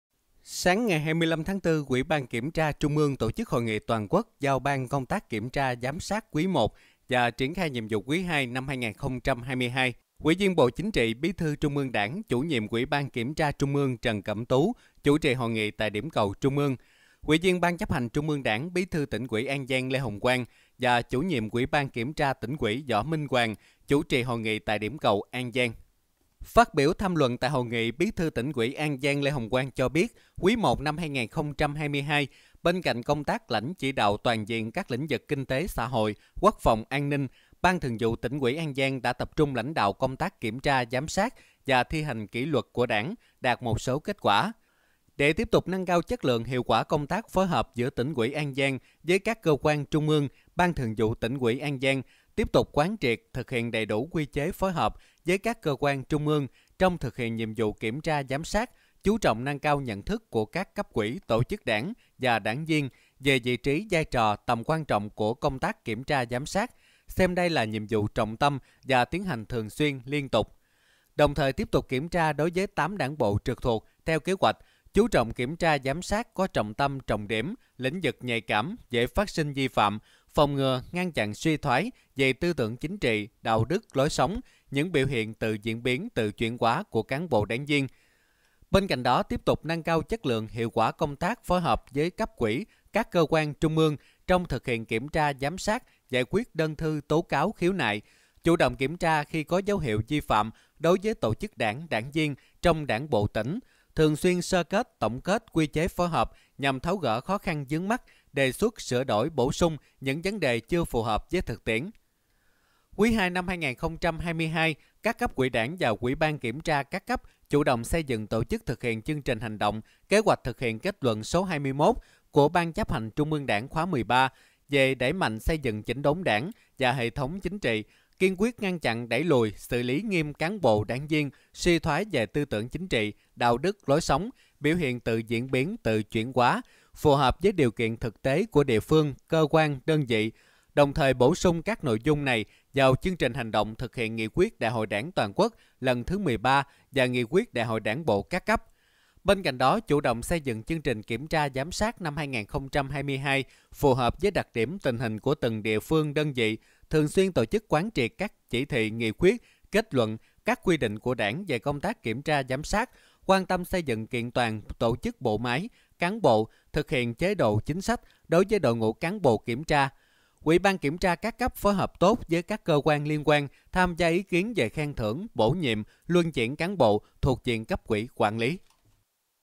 Hội nghị toàn quốc giao ban công tác kiểm tra, giám sát quý I và triển khai nhiệm vụ quý II/2022
Sáng 25/4, Ủy Ban Kiểm tra (UBKT) Trung ương tổ chức hội nghị toàn quốc giao ban công tác kiểm tra, giám sát quý I và triển khai nhiệm vụ quý II/2022.